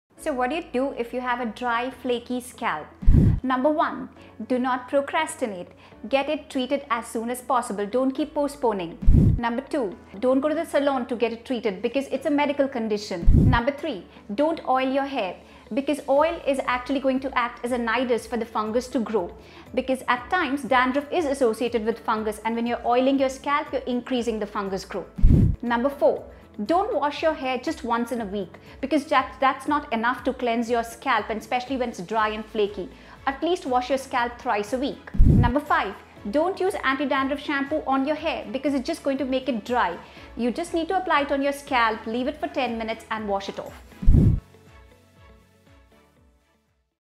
What to do if you have a DRY FLAKY SCALP?
1. Get it treated as soon as possible
2. Don’t go to the salon to get it treated
3. Don’t oil your hair
4. Wash your hair thrice a week
5. Use anti-dandruff shampoo on your scalp (not the hair)

For more updates on skincare and haircare,
follow Dr. DeepikaLunawat on